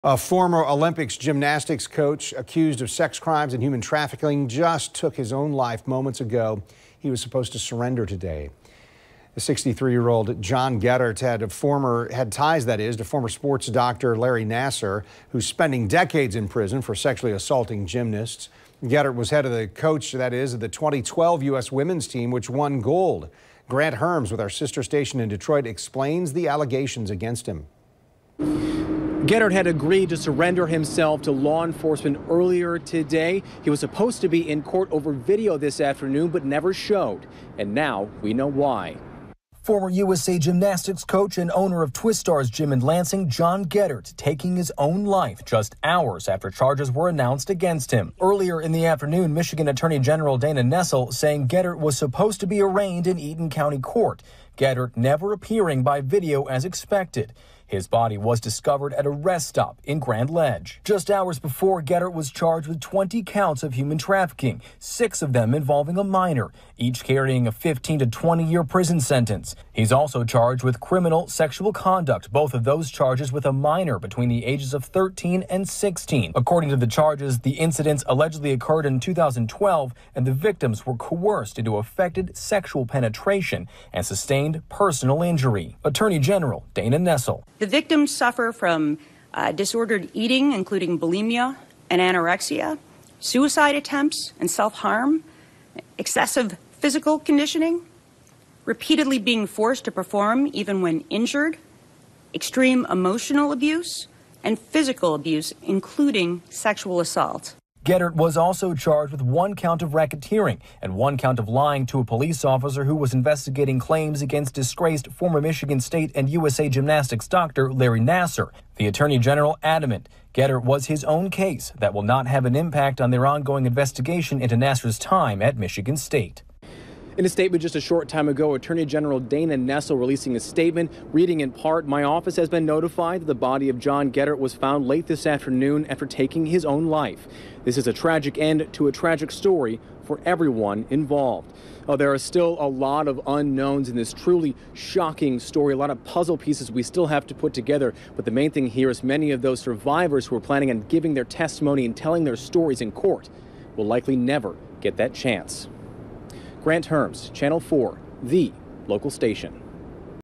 Olympics gymnastics coach dies after being charged
VIDEO: A former Olympics gymnastics coach accused of sex crimes and human trafficking has died.